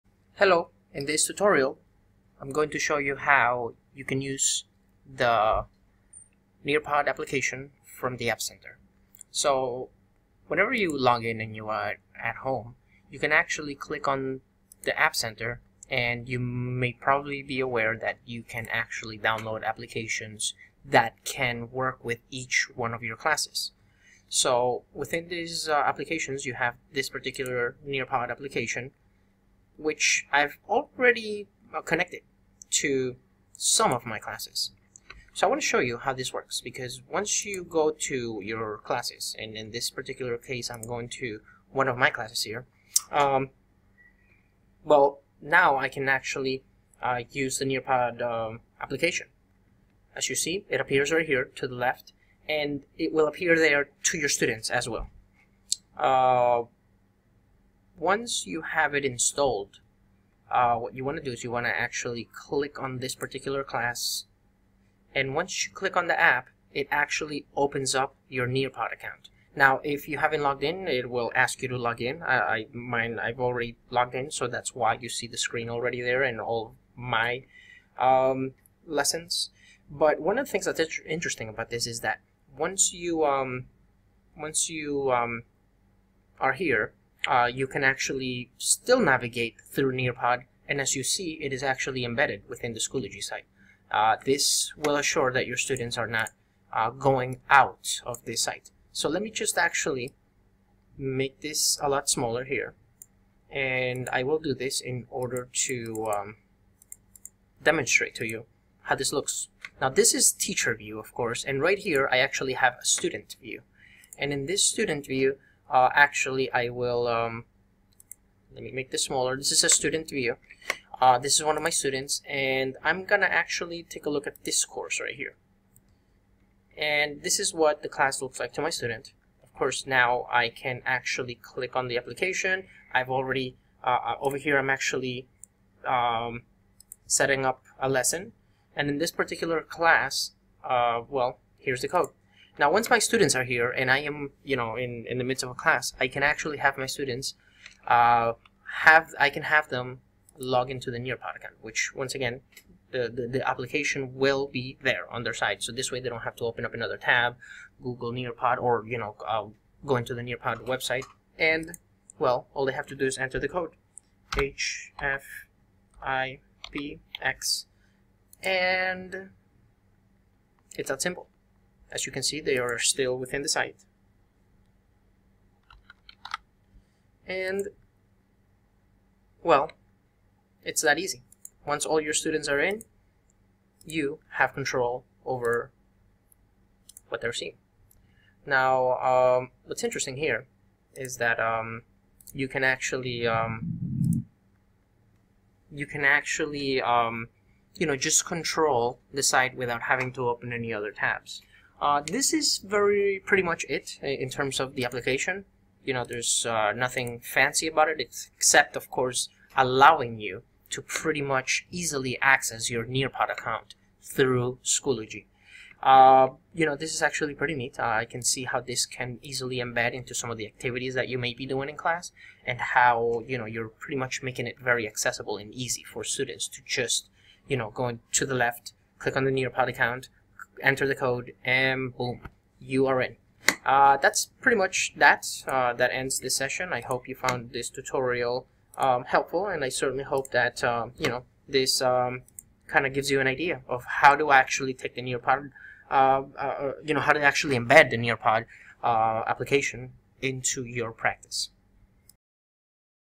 Use the Nearpod app from Schoology
There are various ways that you can embed the app into your Schoology material. Here is one way!